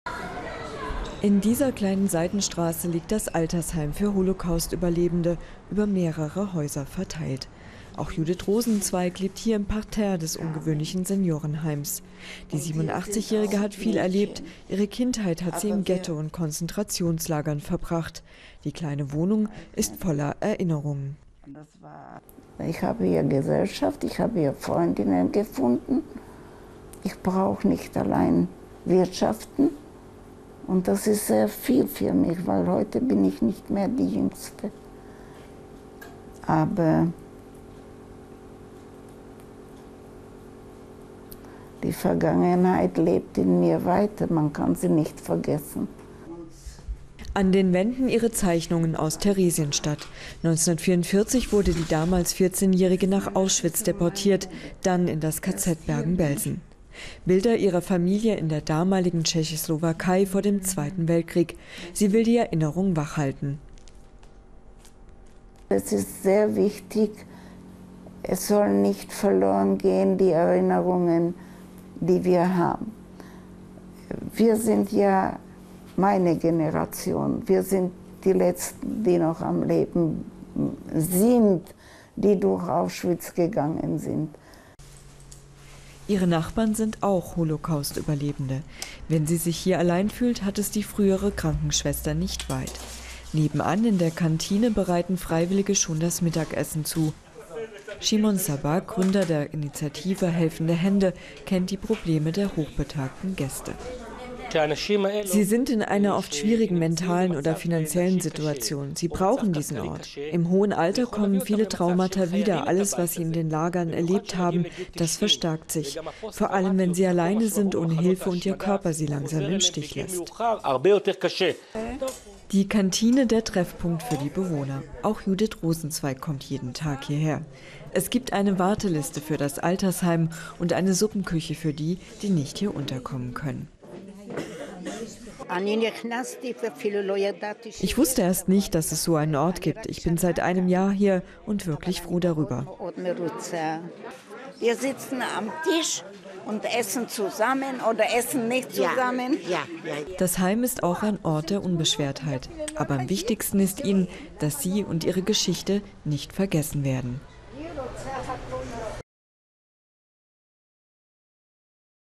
Ein Wohnprojekt für Holocaust-Überlebende | DW Nachrichten
In Israel leben etwa 180.000 Holocaust-Überlebende; einige in einer besonderen Gemeinschaft in Haifa für bedürftige Überlebende. Im Alter quält viele die Vergangenheit stärker als in den Jahren, in denen sie mitten im Leben standen.